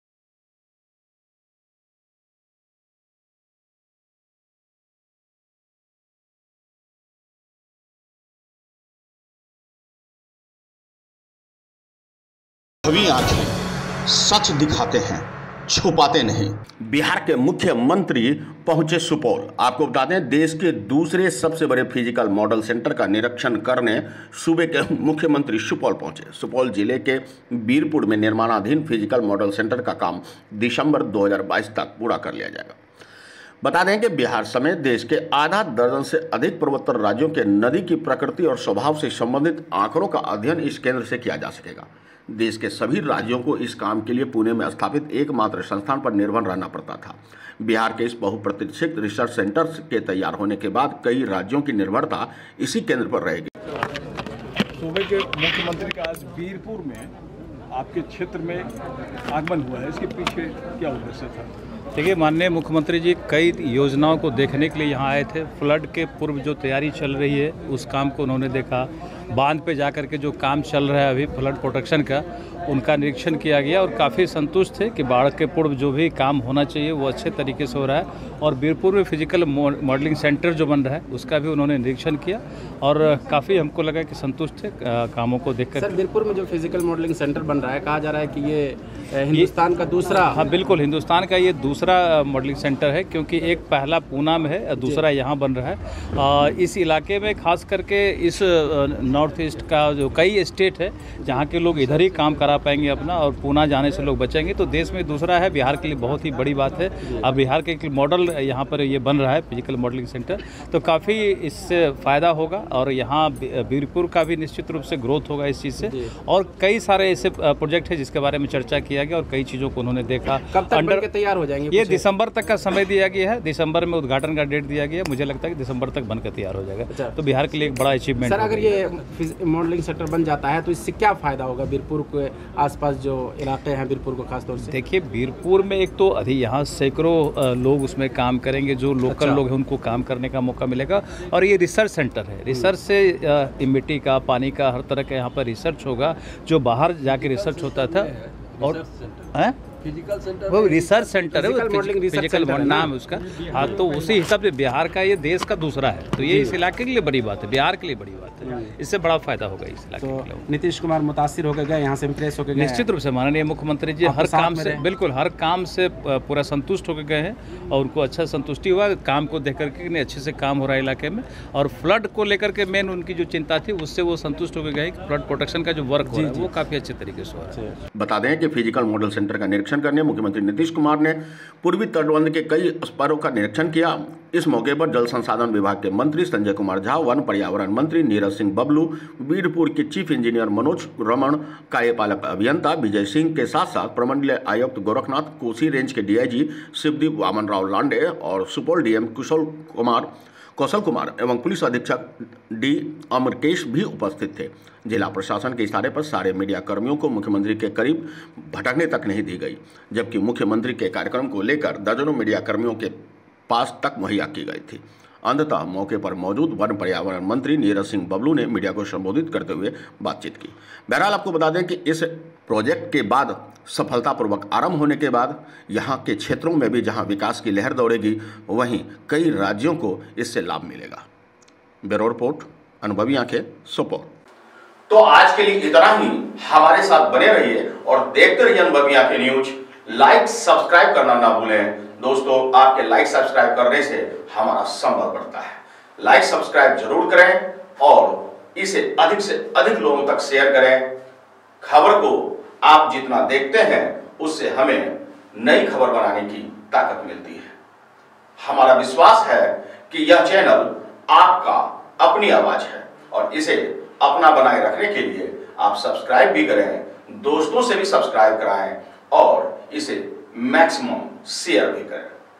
मुख्यमंत्री नीतीश कुमार पहुंचे सुपौल और बिहार को मिला यह गौरव ANUBHAVI AANKHEN
मुख्यमंत्री नीतीश कुमार पहुंचे सुपौल और बिहार को मिला यह गौरव @AnubhaviAankhen